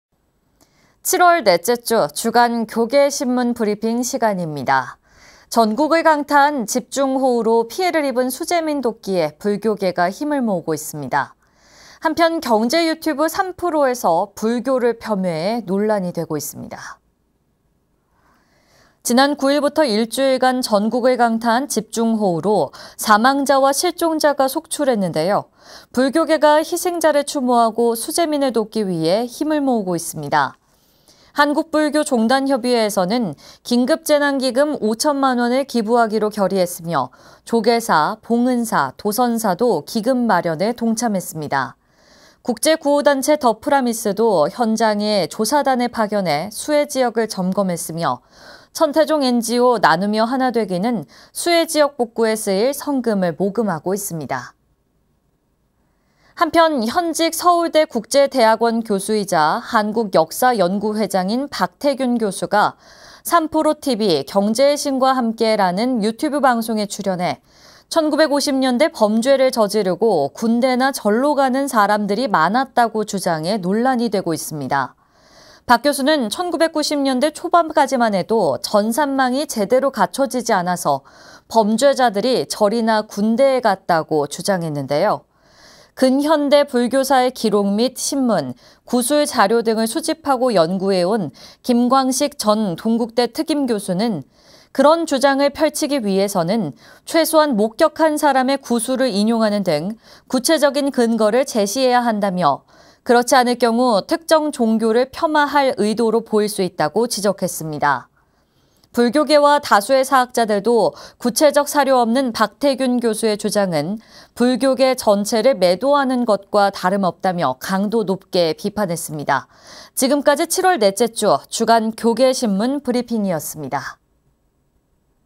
[BTN뉴스] '주간 교계신문 브리핑'
7월 넷째 주 ‘주간 교계신문 브리핑’ 시간입니다. 전국을 강타한 집중호우로 피해를 입은 수재민 돕기에 불교계가 힘을 모으고 있습니다. 한편, 경제 유튜브 ‘3프로’에서 불교를 폄훼해 논란이 되고 있습니다.

지난 9일부터 일주일간 전국을 강타한 집중호우로 사망자와 실종자가 속출했는데요. 불교계가 희생자를 추모하고 수재민을 돕기 위해 힘을 모으고 있습니다.

한국불교종단협의회에서는 긴급재난 기금 5천만원을 기부하기로 결의했으며, 조계사, 봉은사, 도선사도 기금마련에 동참했습니다.

국제구호단체 더프라미스도 현장에 조사단을 파견해 수해지역을 점검했으며, 천태종 NGO 나누며하나되기는 수해지역 복구에 쓰일 성금을 모금하고 있습니다.

한편, 현직 서울대 국제대학원 교수이자 한국역사연구회장인 박태균 교수가 ‘3프로 TV-경제의 신과 함께’라는 유튜브 방송에 출연해 “1950년대 범죄를 저지르고, 군대나 절로 가는 사람들이 많았다”고 주장해 논란이 되고 있습니다.

박교수는 ‘1990년대 초반까지만 해도 전산망이 제대로 갖춰지지 않아서 범죄자들이 절이나 군대에 갔다.’고 주장했는데요.

근현대불교사의 기록 및 신문, 구술자료 등을 수집하고 연구해온 김광식 전 동국대 특임교수는 ‘그런 주장을 펼치기 위해서는 최소한 목격한 사람의 구술을 인용하는 등 구체적인 근거를 제시해야 한다며 그렇지 않을 경우 특정 종교를 폄하할 의도로 보일 수 있다.’고 지적했습니다.

불교계와 다수의 사학자들도 구체적 사료 없는 박태균교수의 주장은 불교계 전체를 매도하는 것과 다름없다며, 강도 높게 비판했습니다.

지금까지 7월 넷째 주 ‘주간 교계신문 브리핑’이었습니다.